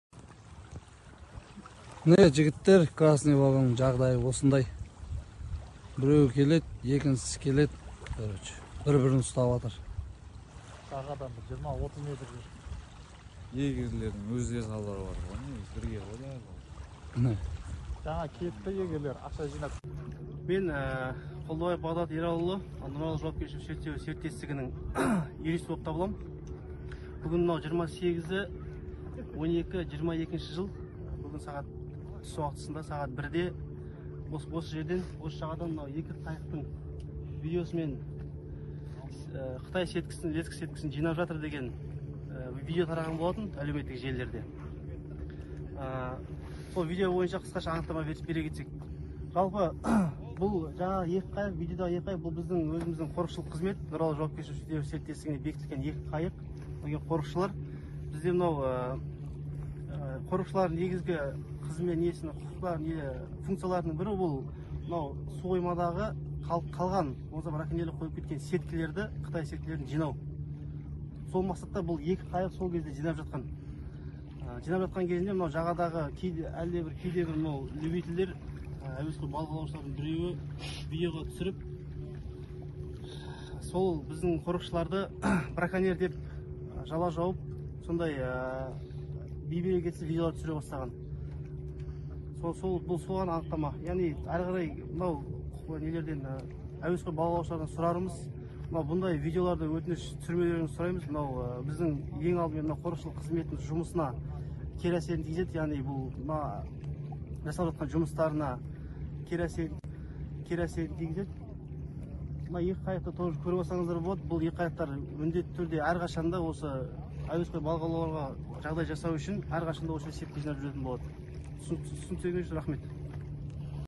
Интернетті шулатқан видео || Шардара 2023
Интернетті шулатқан видео
Шардара 2023 Браконьер 2023
Браконьерлермен күрес
Шардара су қоймасын браконьерлердің құрған ауларынан тазарту жұмыстары.
Очистка Шардаринского водохранилища от сетей, расставленных браконьерами.

Объяснение управления Шардаринским водохранилищем видеороликам, вызвавшим ажиотаж в интернете.